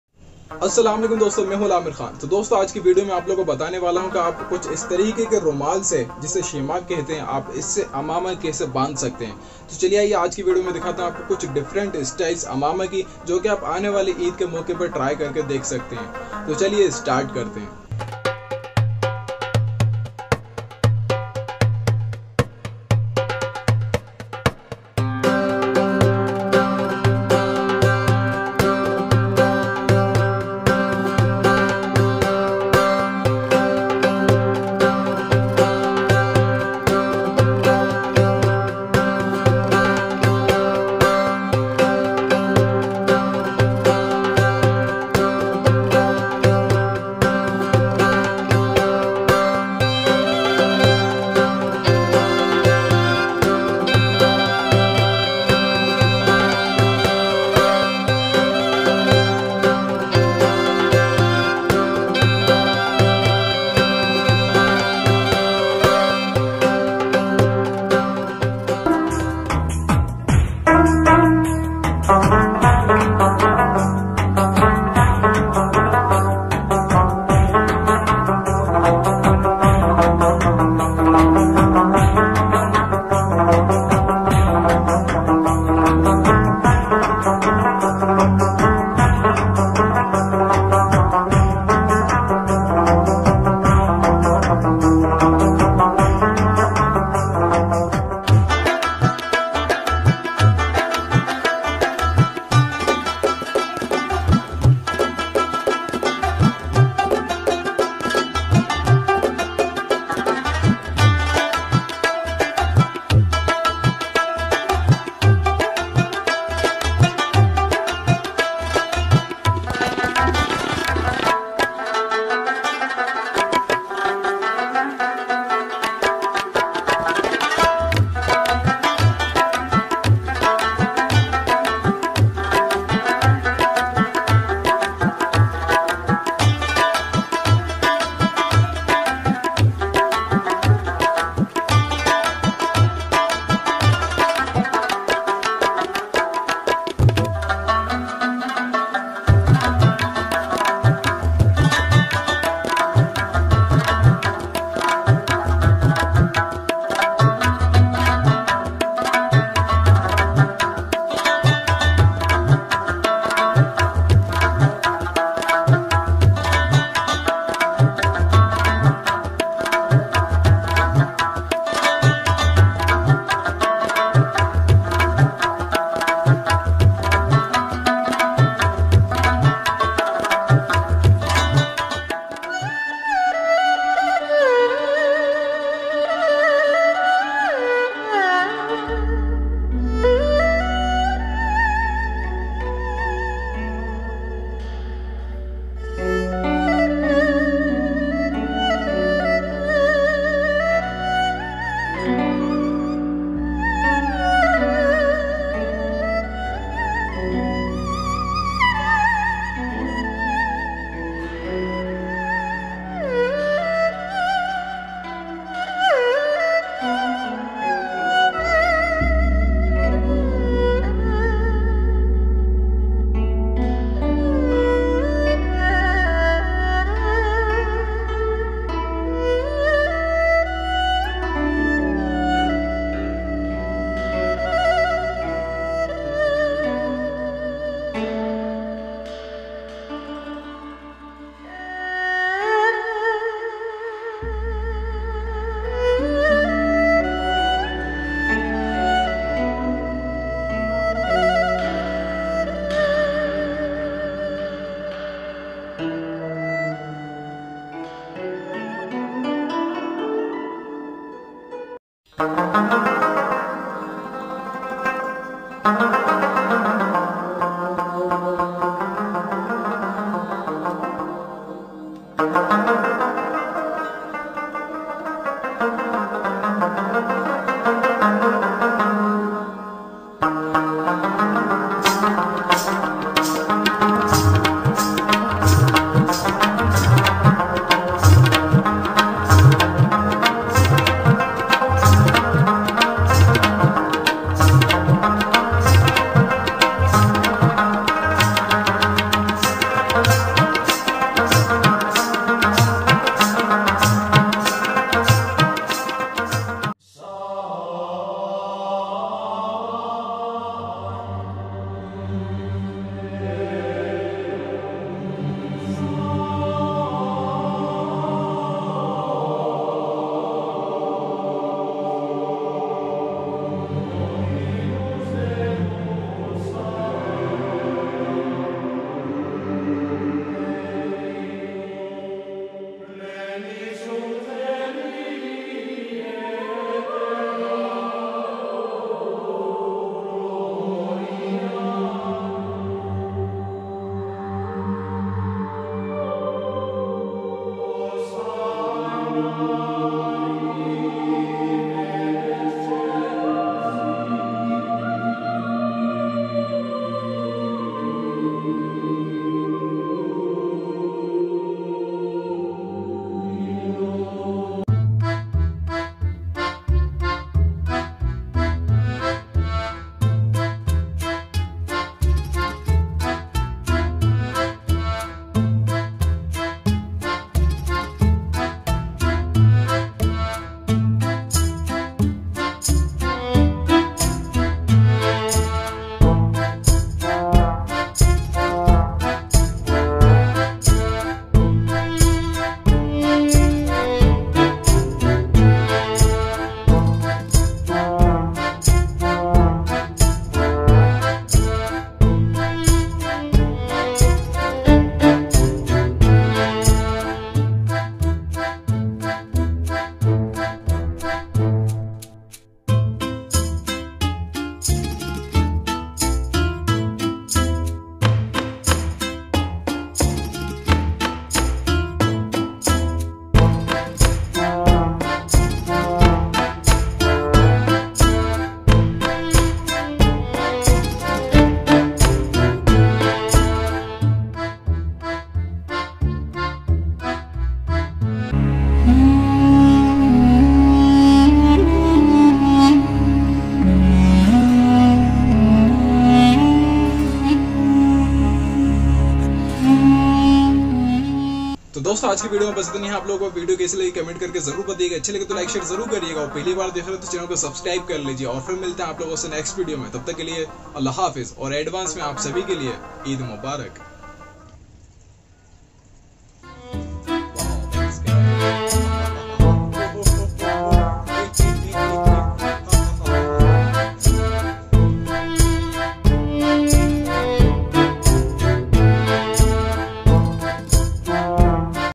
Eid Special - How to Tie AMAMAH Shemagh Scarf | IMAMA Different Styles Tutorial | AL Aamir Khan
In this video I am showing different styles of Amma sharif and how can you tie Amamah Shareef with Shemagh scarf. and wearing Amamah is a sunnah we use it specially on Eid.

How to tie scarf tutorial -
Amamah, IMAMA, amama, different Style to wear Shemagh Scarf, 2022 Eid special video

Thanks
Timelapse:
00:00 - Intro
00:22 - Style 1
01:09 - Style 2
01:39 - Style 3
03:02 - Style 4
04:22 - Style 5
05:07 - Style 6
06:05 - Style 7